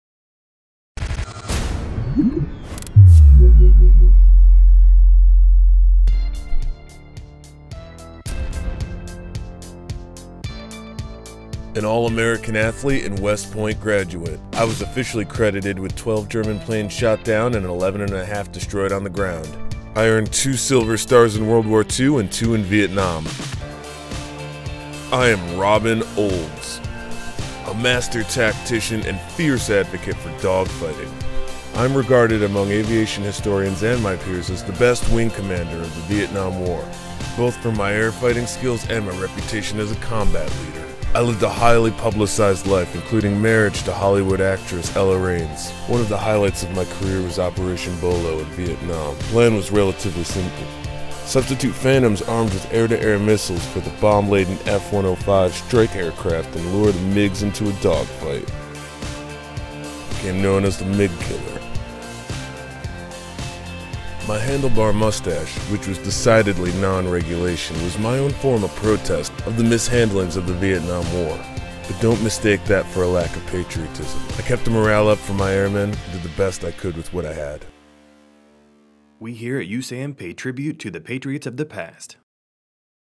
Patriots Of The Past: Robin Olds
Robin Olds was an American Fighter Pilot and Triple Ace of the U.S. Air Force.